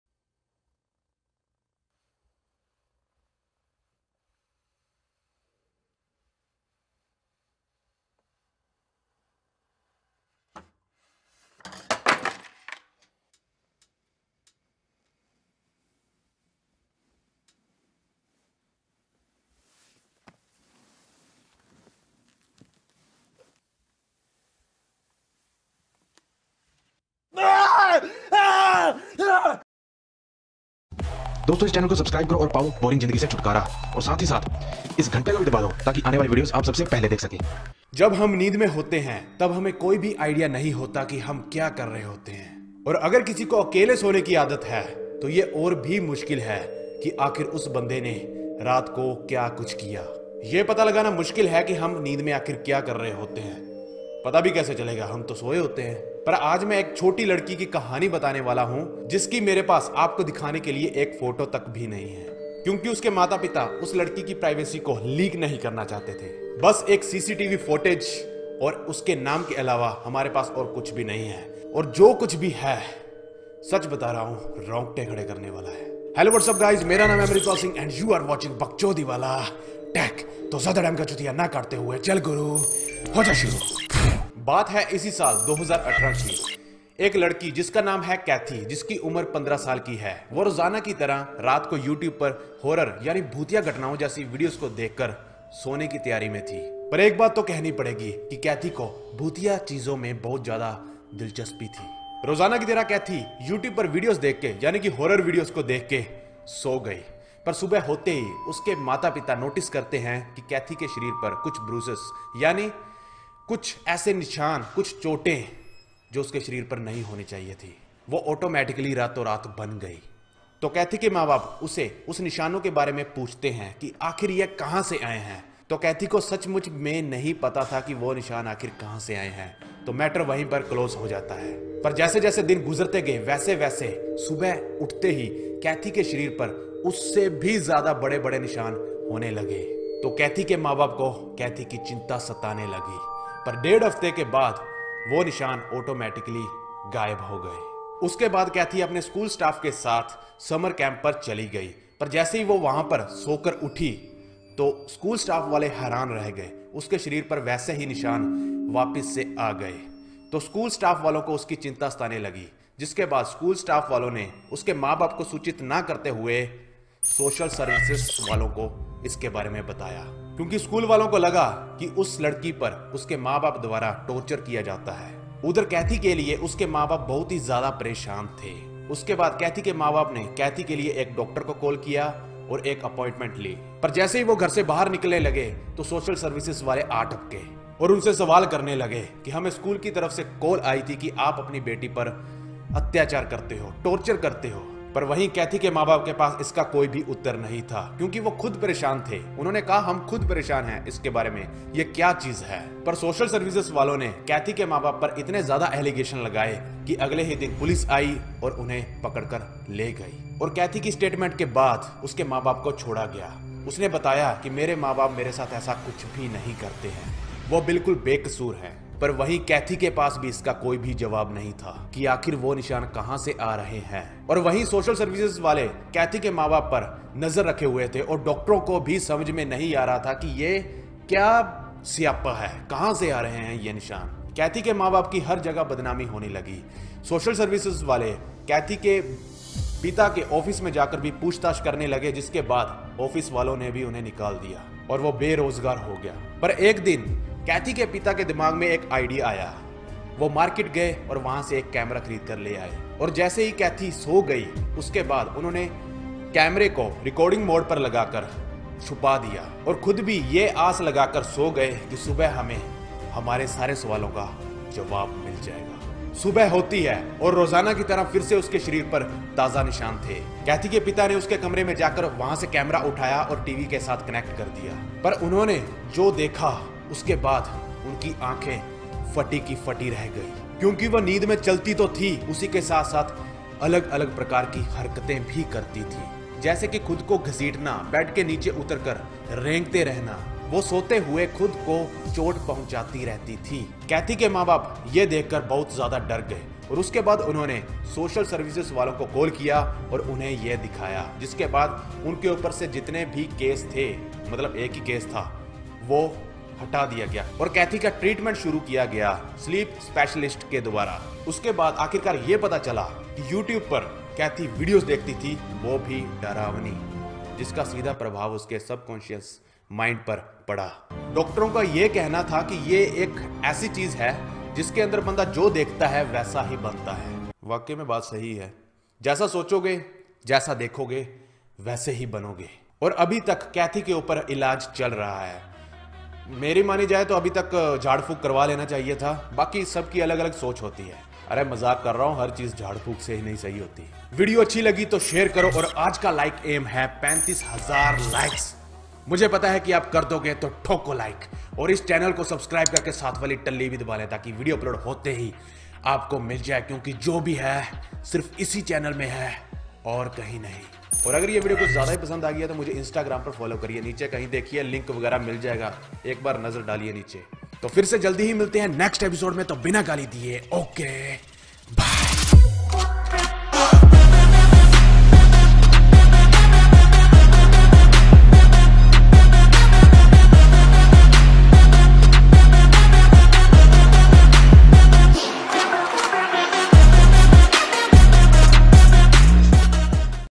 होश उड़ा देने वाली Video | Paranormal activity | The Science of Ghosts
Have you ever seen a ghost or experienced something paranormal? Greg Foot takes a look at what science has to say about ghosts.